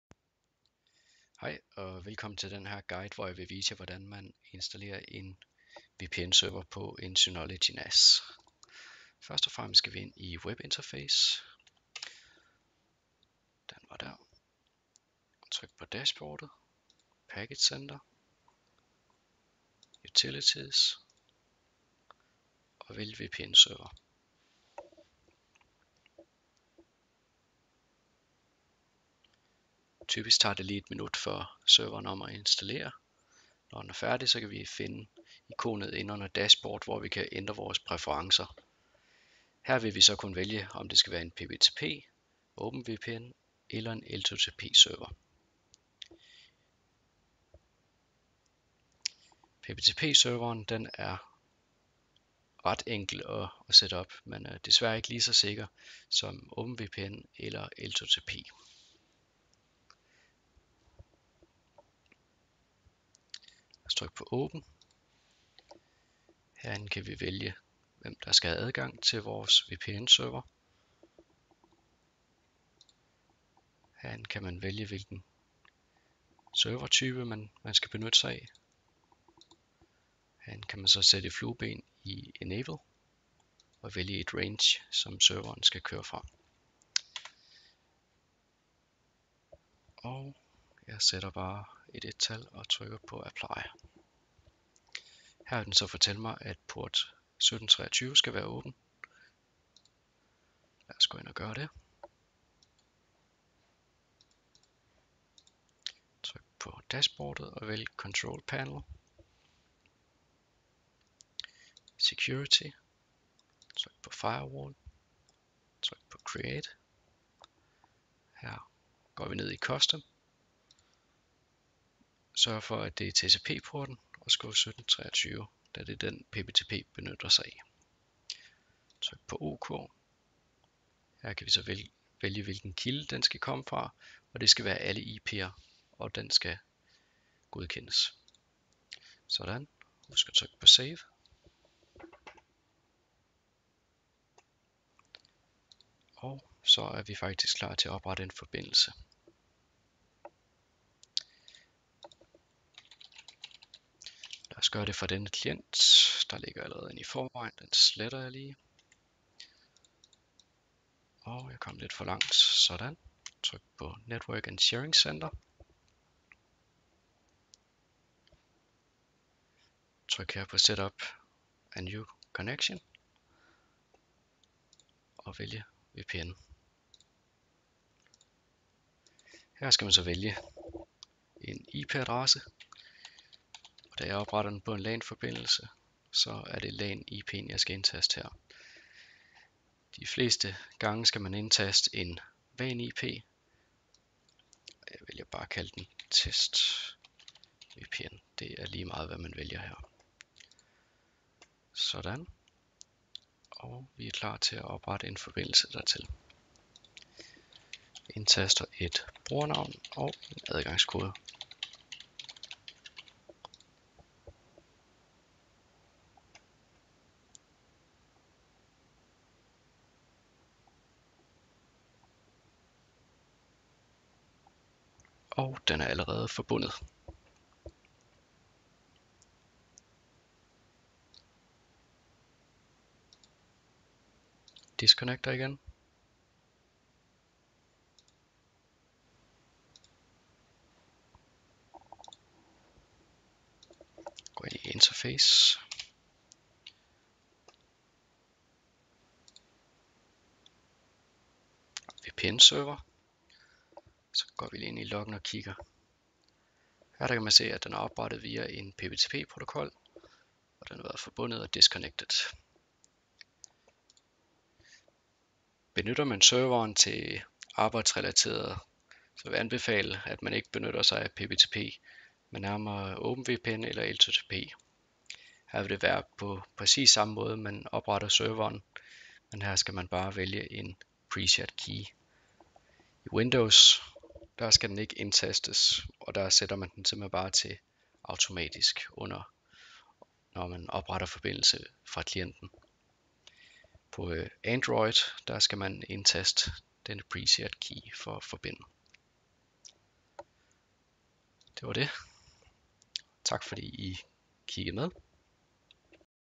Sådan oprettes en VPN-forbindelse på Synology NAS [ synoguides.dk ]
VPN videoguide til installation af VPN-server samt oprettelse af forbindelse fra client.